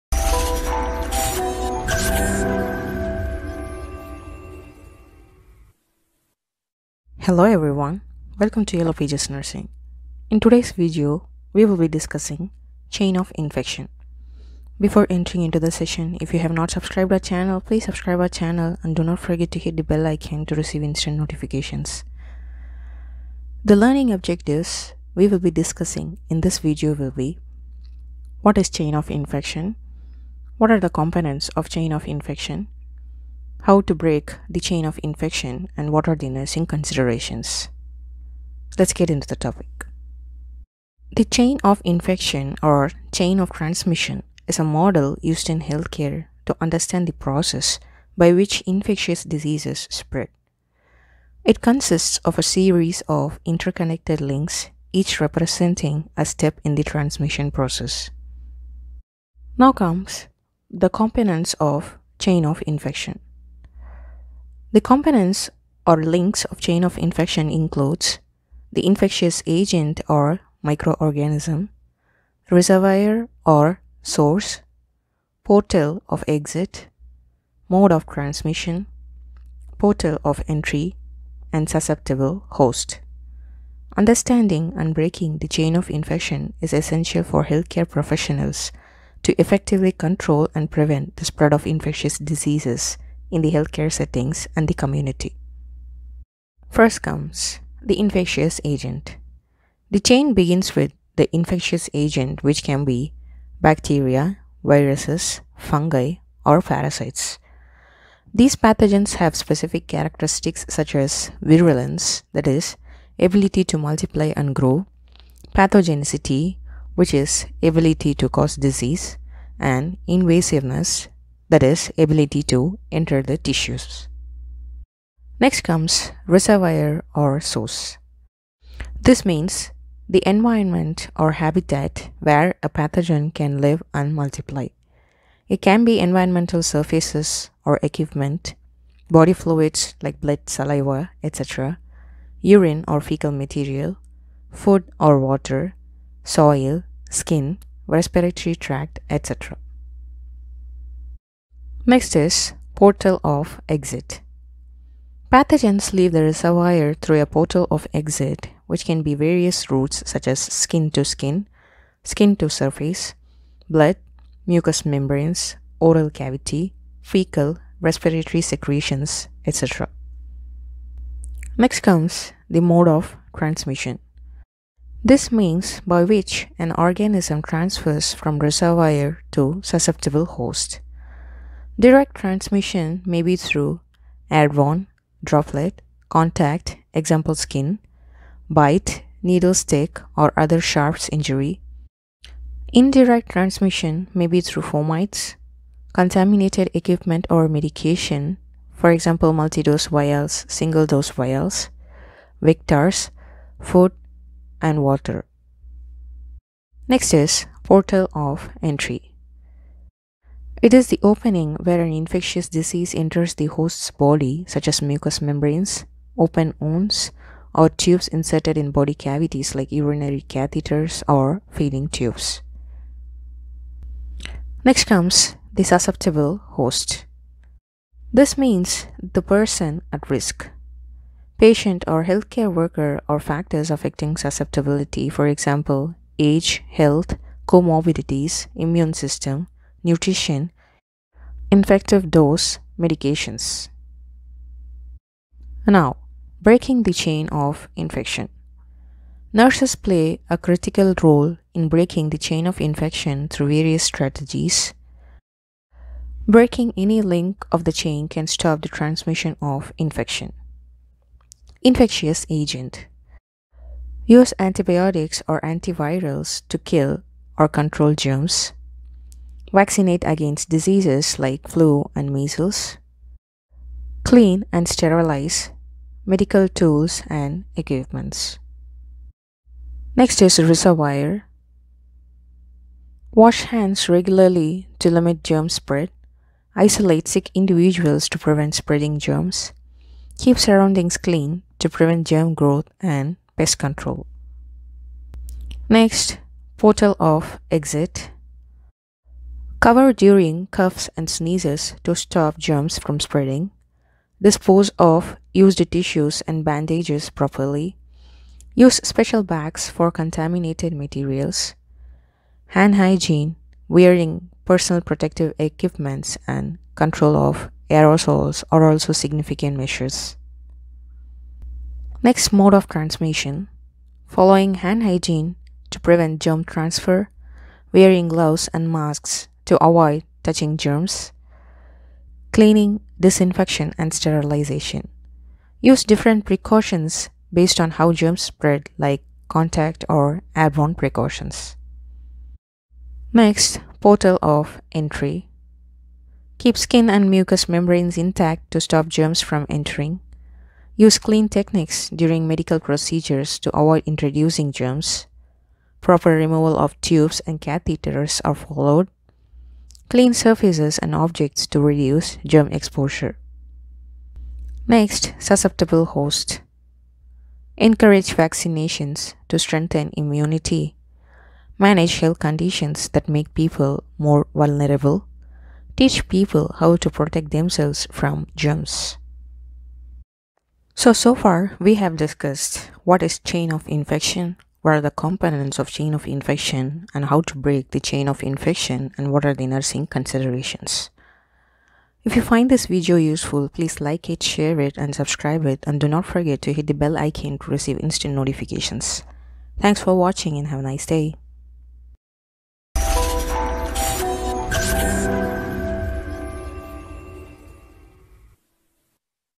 Chain of infection | Break the chain of infection | Made Easy | Nursing
What is chain of infection?,
What are the components of chain of infection?,
How to break the Chain of Infection ?,
What are the nursing considerations?,
The COMPONENTS OR  links OF chain of infection include:
the infectious agent OR MICRO ORGANISM,
Reservoir/SOURCE,
Portal of exit,
Mode of transmission,
Portal of entry,
Susceptible host.

     Yellow pages nursing (YPN) attests to the accuracy of the information contained here but takes no responsibility for how it may apply to an individual patient.
      The following content in this video is for informational use and not intended to set or be a replacement of your hospital protocols or procedures.